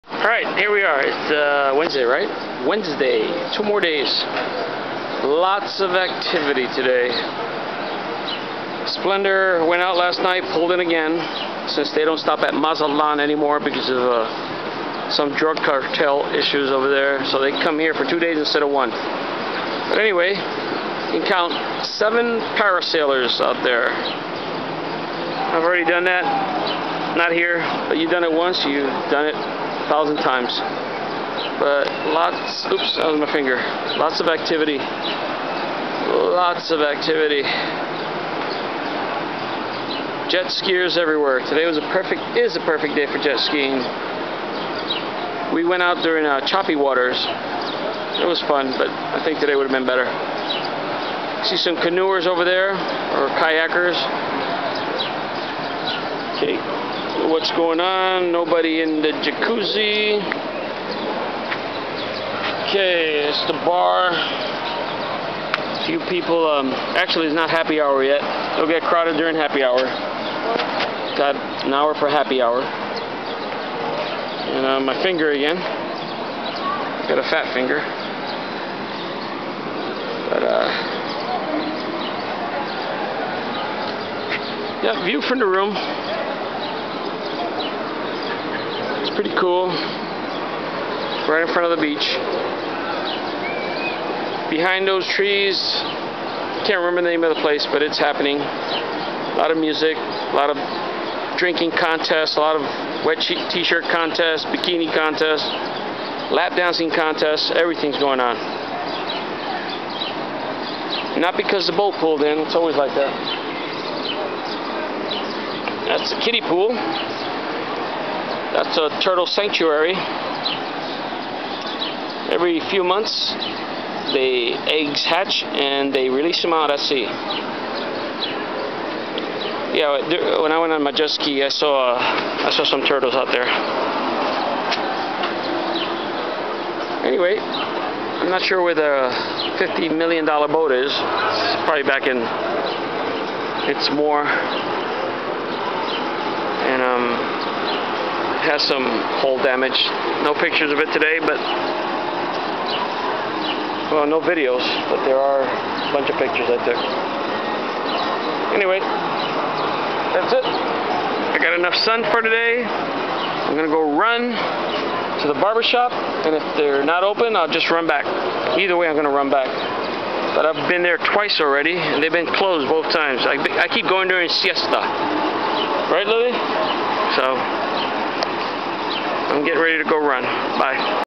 View from room at Casa Dorado at Cabo San Lucas Day 4
Here is another view from the room my parents took at Casa Dorado in Cabo San Lucas, Mexico.